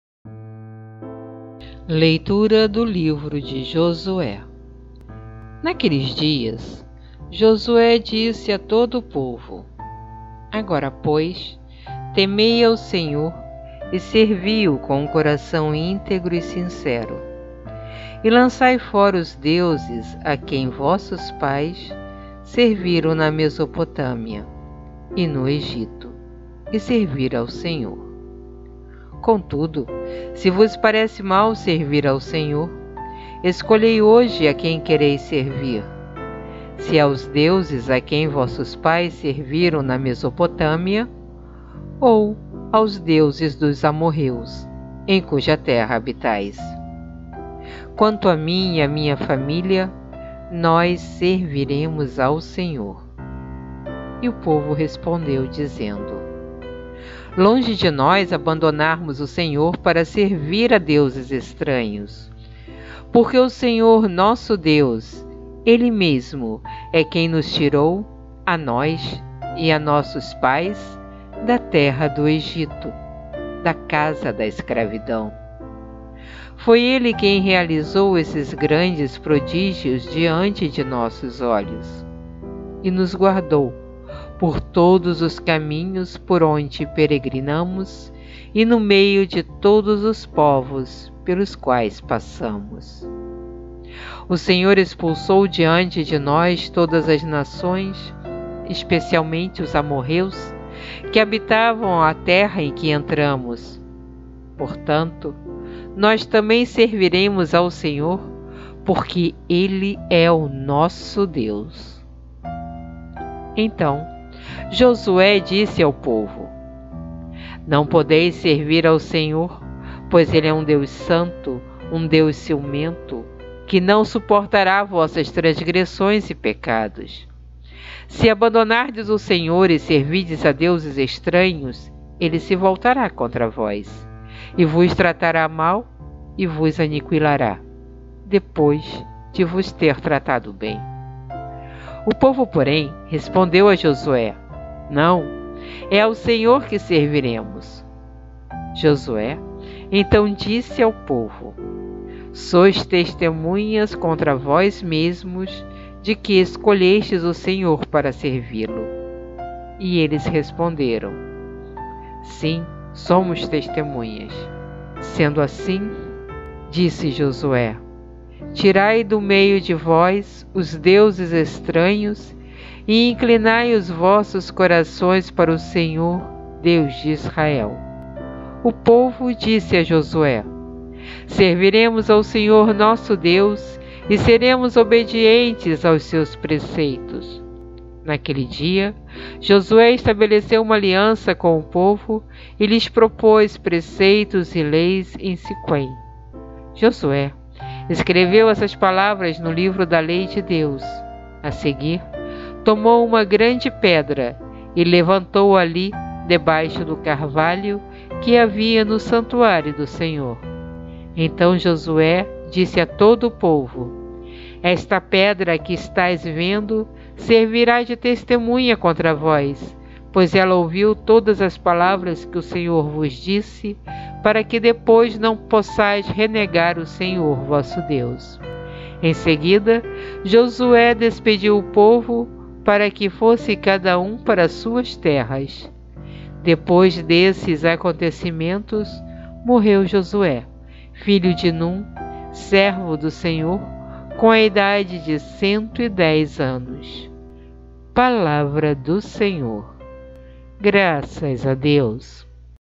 (17/08/2019) Sábado - Leitura 19ª Semana do Tempo Comum
Leitura do Livro de Josué capítulo 24 versículos de 14 a 29

2019-17/08

Esse vídeo é destinado para todos aqueles que desejam acompanhar a primeira leitura da missa diária.

Artista: Wahneta Meixsell

Imagem de OpenClipart-Vectors por Pixabay
Imagem de Jill Wellington por Pixabay